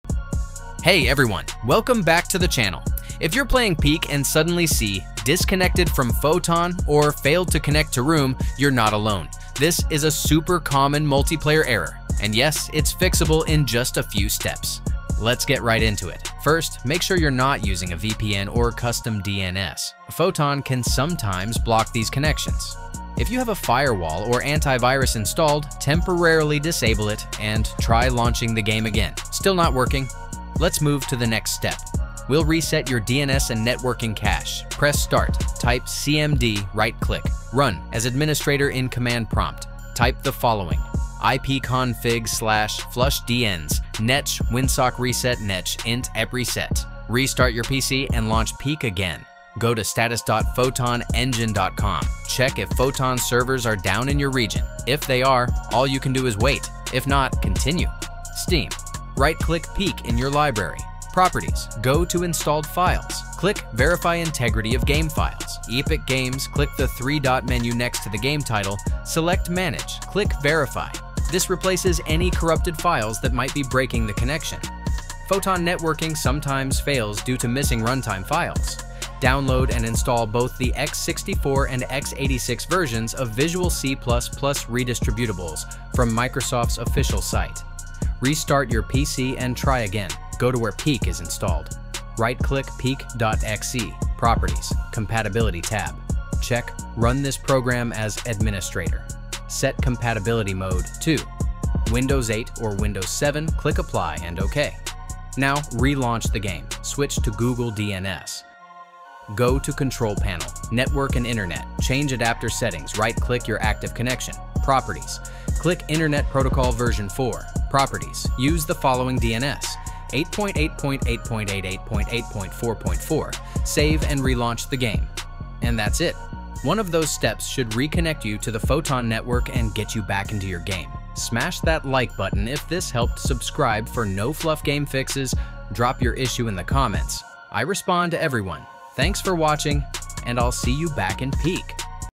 Fix PEAK “Disconnected from Photon” Error | Easy Network Fix (2025)
In this quick tutorial, I’ll show you how to fix the “Disconnected from Photon” or “Failed to Connect to Photon Network” errors in PEAK.
These connection issues usually happen because of firewall blocks, DNS errors, or broken game files. Follow this step-by-step guide to get back online and reconnect to the Photon servers successfully.

🛠️ Issues addressed in this video:

Disconnected from Photon in PEAK

Failed to connect to Photon server or room

Multiplayer not working in PEAK

PEAK stuck on “Connecting” or “Room not found”

✅ Works on Windows 10, Windows 11 — PC, Laptop/Desktop.